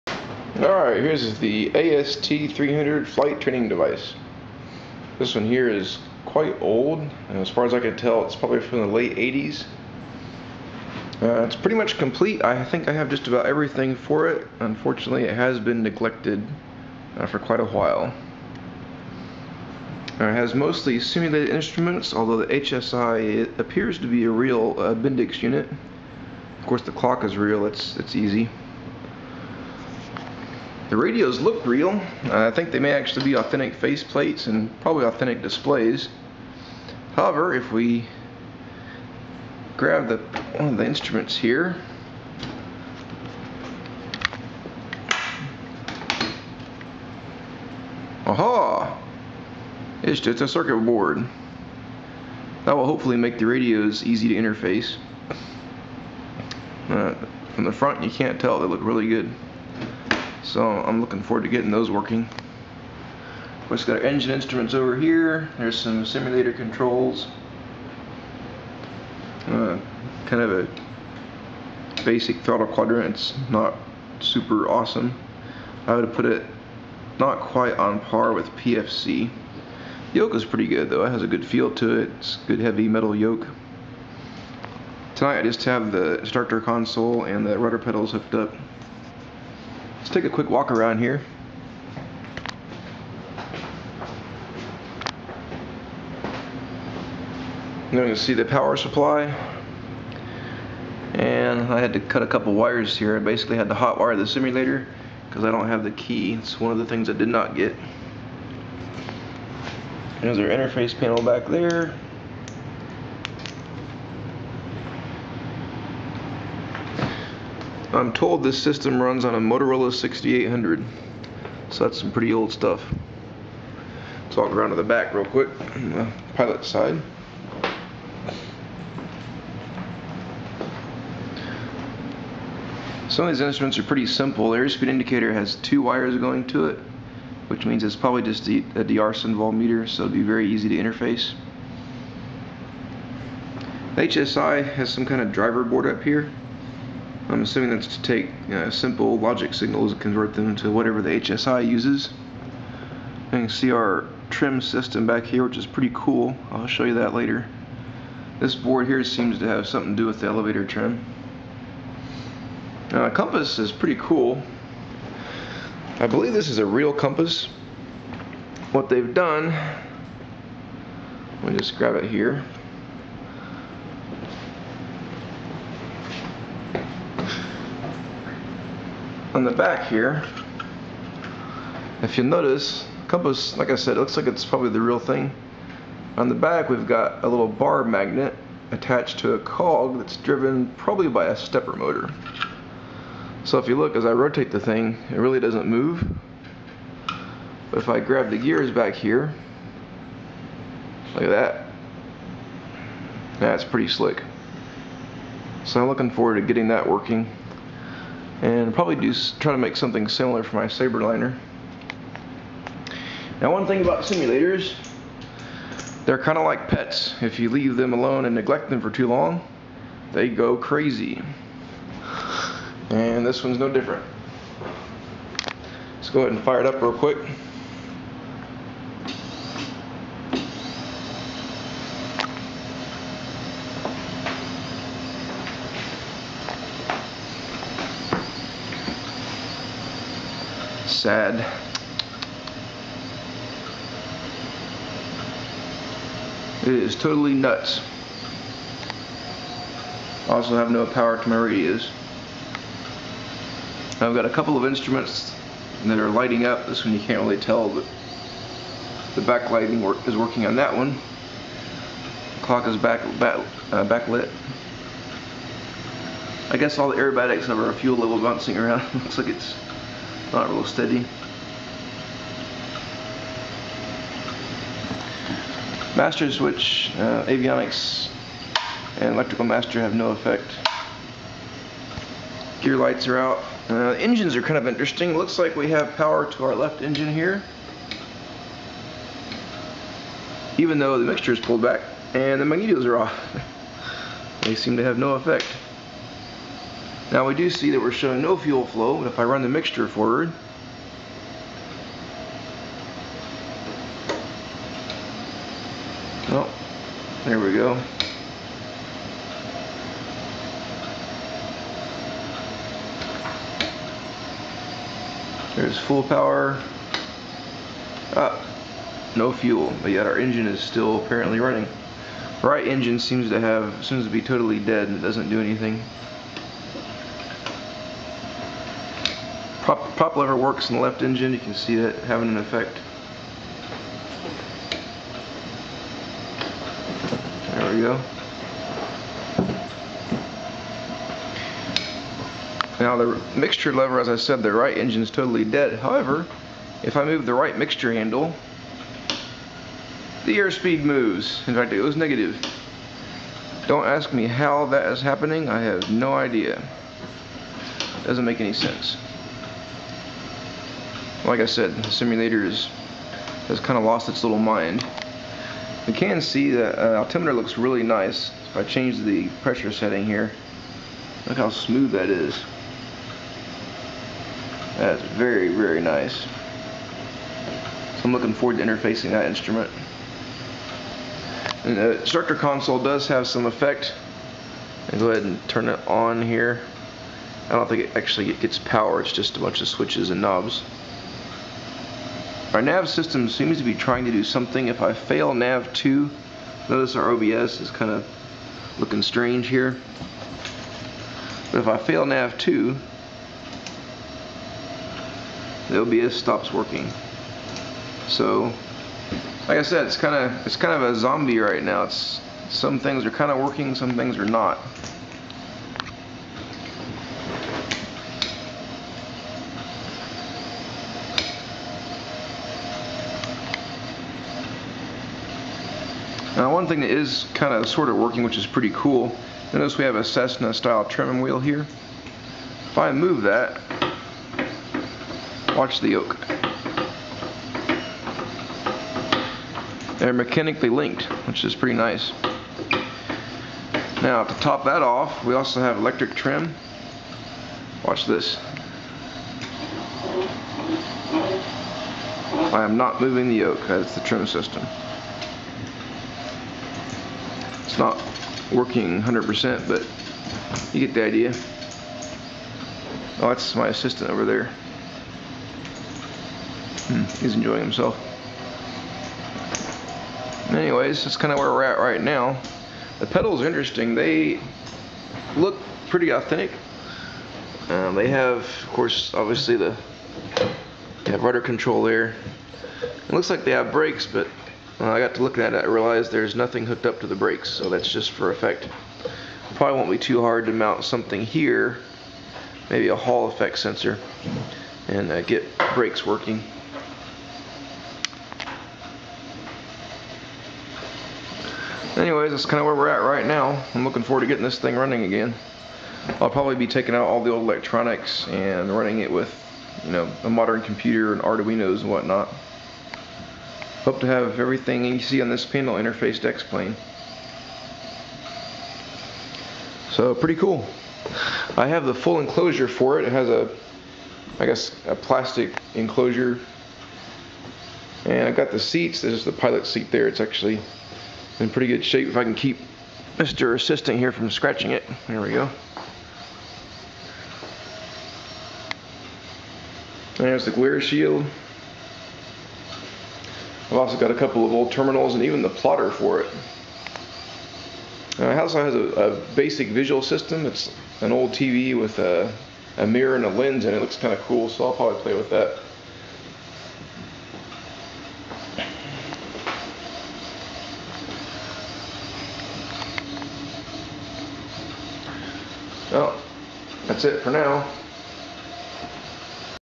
AST-300 flight simulator
Here's a cool little project I picked up yesterday. This is a very complete, but long-neglected AST-300 flight training device. It mounts in an enclosure, but the real goodies are mounted in a self-contained metal frame with the instrument panel, as seen here. This entire unit drops onto the enclosure and is held in place with four bolts.

I will at some point start interfacing all the instruments. I may use the King avionics stack in another sim (based on a real cockpit), and maybe some of the instruments, but for now I will probably leave the sim together and make it "fly" in its current configuration (with new electronics, of course).